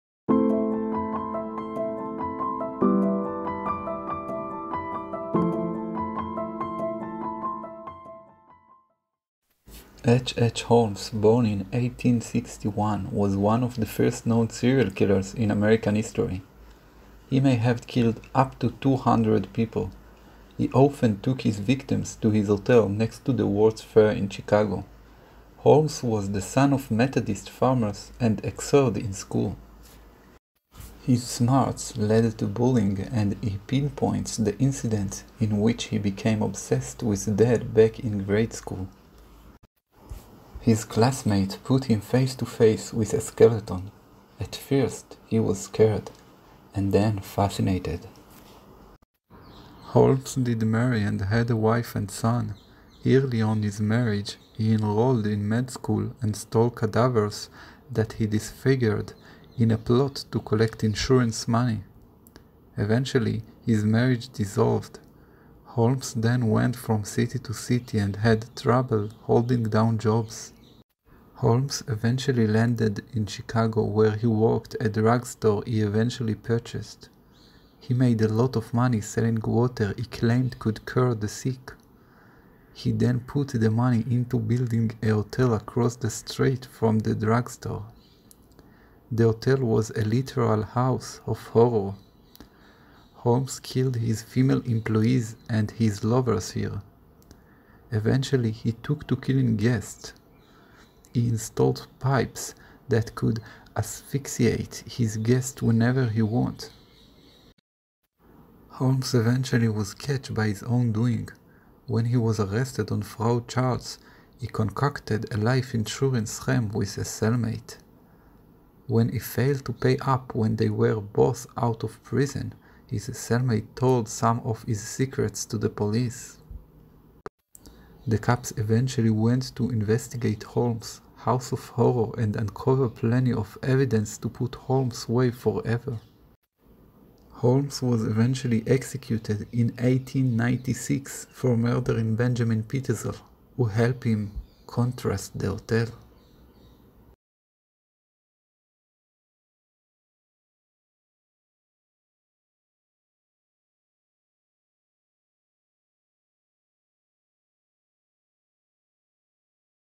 H H Holmes american horror story
H H Holmes american horror story about 1 of the first serial killer in american history.
Holmes build his own hotel with the right structure to help him kill his victims.